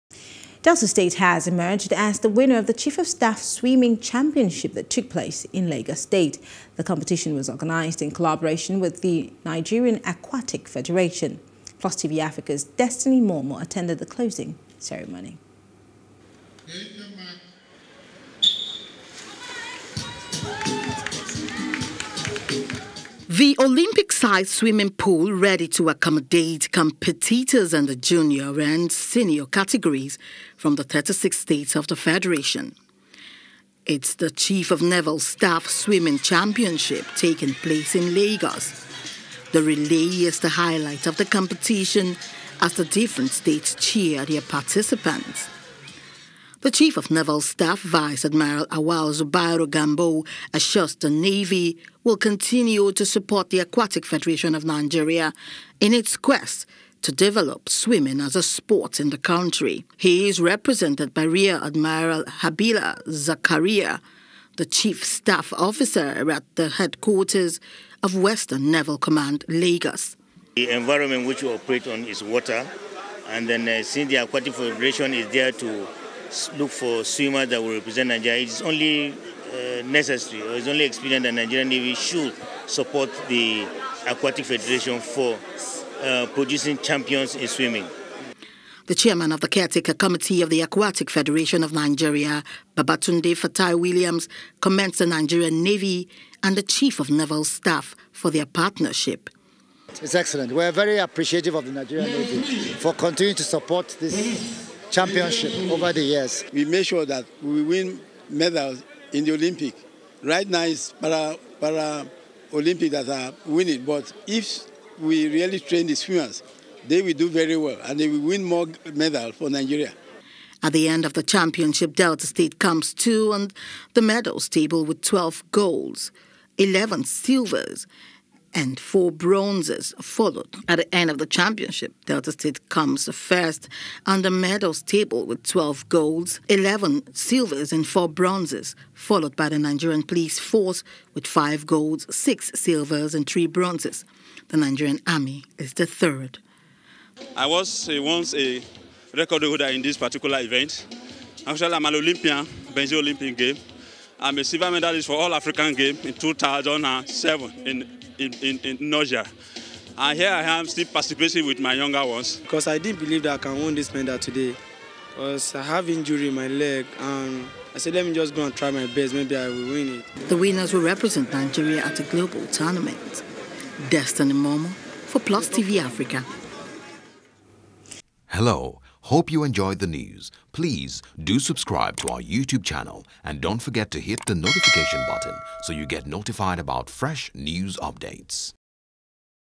Navy Swimming Championship: Delta State Emerges Winner Of Chief Of Naval Staff Competition | SPORTS
Delta state has emerged the winner of the Chief of Staff swimming championship that took place in Lagos State.